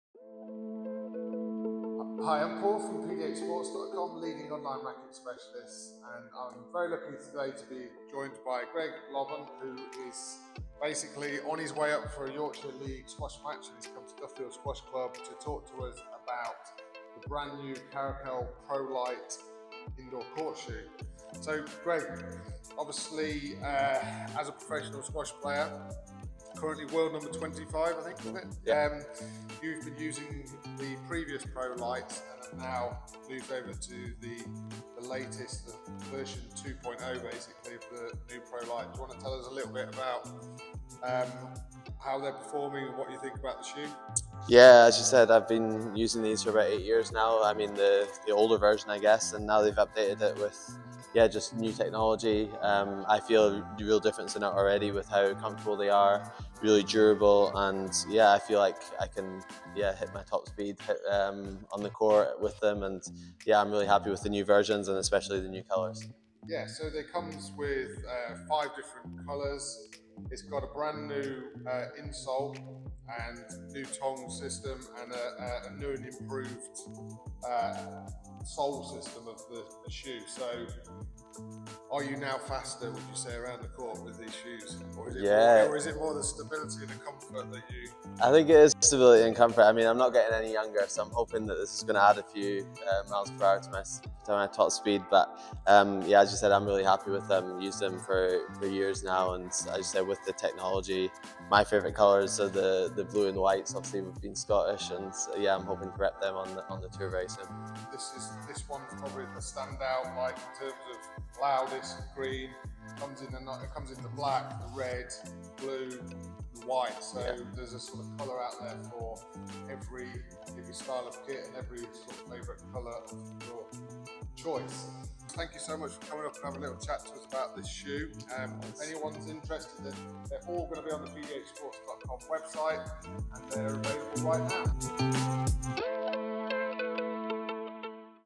Greg Lobban reviews the Karakal KF ProLite Shoe with pdhsports.com
New review of the Karakal KF ProLite squash shoes filmed with professional squash player Greg Lobban. These shoes are available at pdhsports.com now and also in our Derbyshire showroom.
The latest shoe features:
an updated designed for great comfort
lace loop system for reinforced lateral support
shock absorbing Gel Insole
Microfibre material helps your feet stay cool
Please note these shoes are a relatively small fit - we recommend going up a size, especially if you are in between sizes.